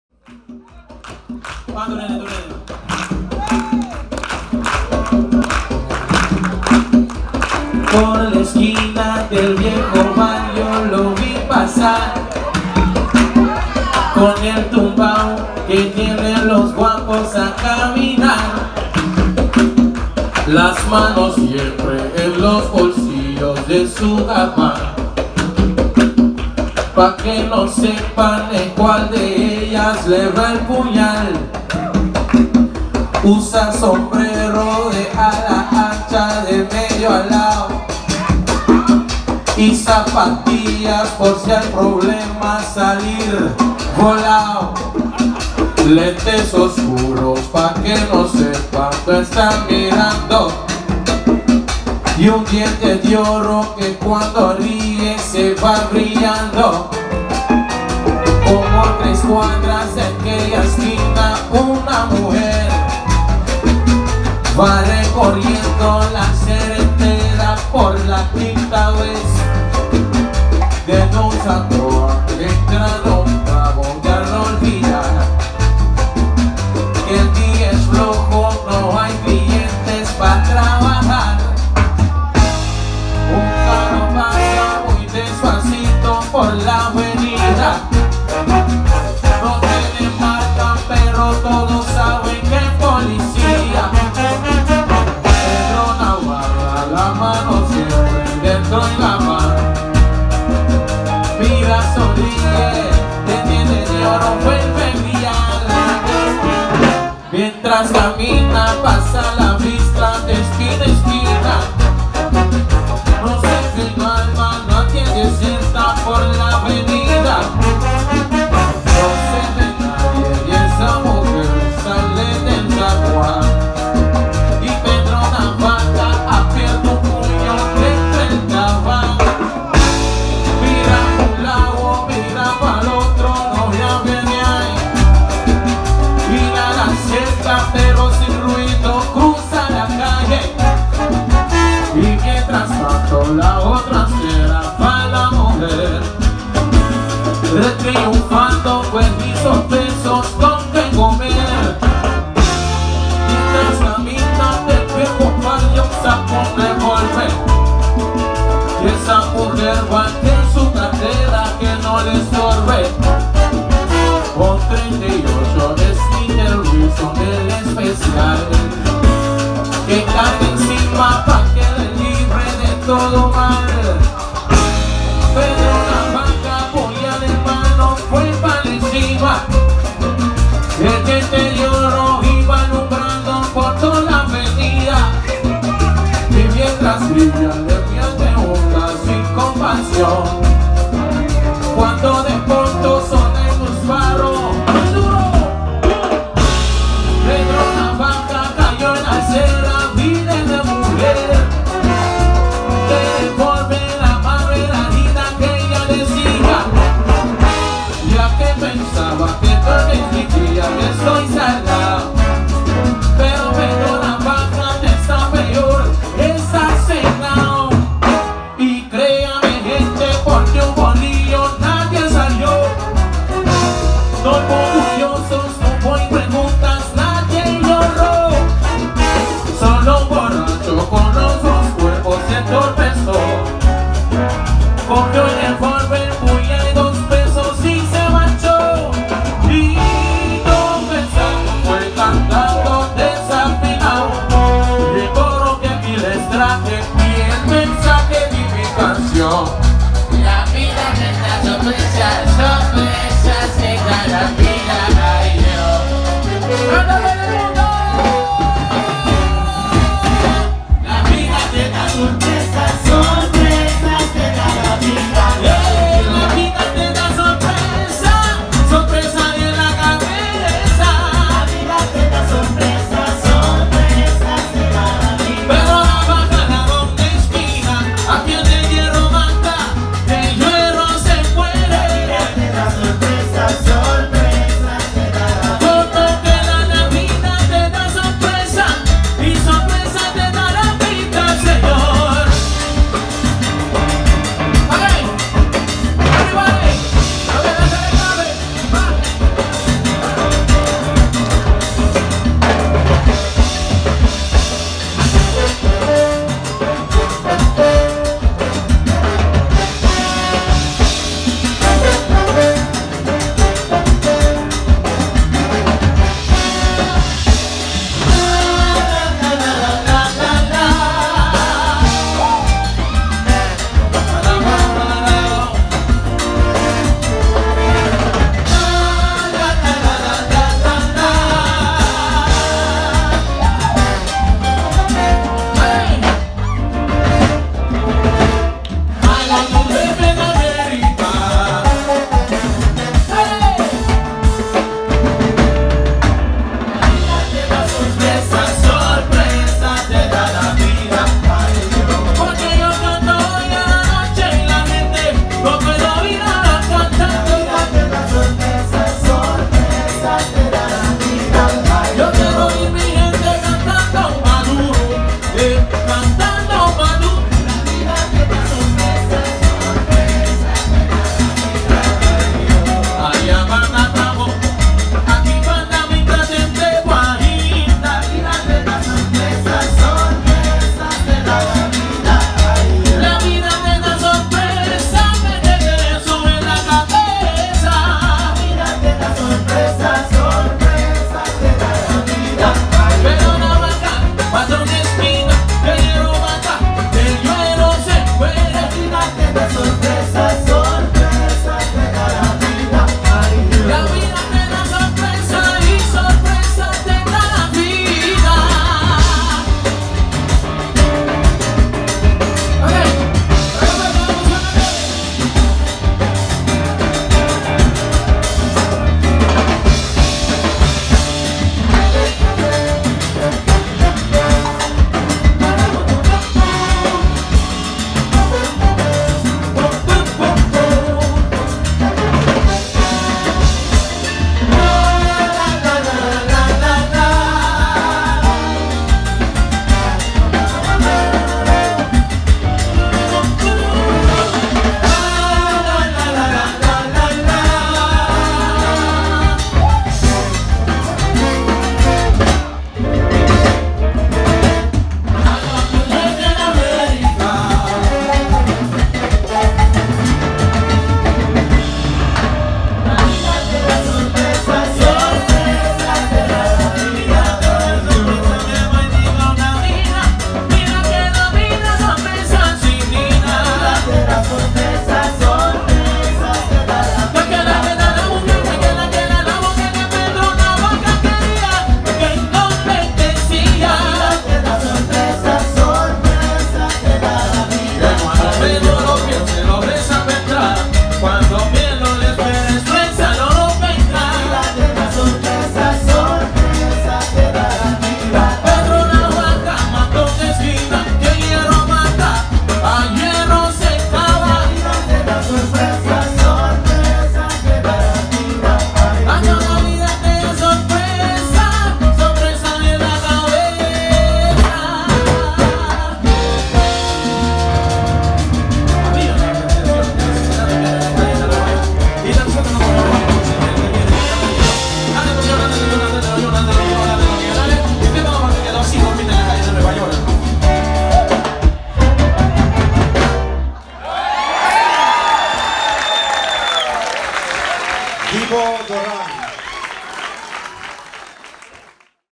Salsa de los Bravos (1)
Salsa uit New York. Salsa de los Bravos bij Brakkeput Mei Mei, Curaçao (1)

Pianist en bandleider Randal Corsen en presenteert Salsa in de stijl van ‘Salsa de los Bravos’.

Deze keer werd het accent gelegd op salsa, meer specifiek op de salsa uit New York. De ‘salsa brava’, kortom vrij wild, om niet zeggen authentiek, die grote artiesten in de salsa zo voortreffelijk voor het voetlicht hebben gebracht als Ruben Blades, Hector Lavoe, Cheo Feliciano, Celia Cruz. Eddie Palmieri en Ray Barreto.

Randal Corsen heeft als muzikaal leider van de avond een orkest samengesteld van 17 uitstekende musici, onder wie blazers en een ritmesectie als aanvulling op zijn vaste groep van muzikale vrienden. De salsa nummers worden die avond vertolkt door de zangers Dibo Doran, Yumarya Grijt i Rich Pieter 'Richie Rich’.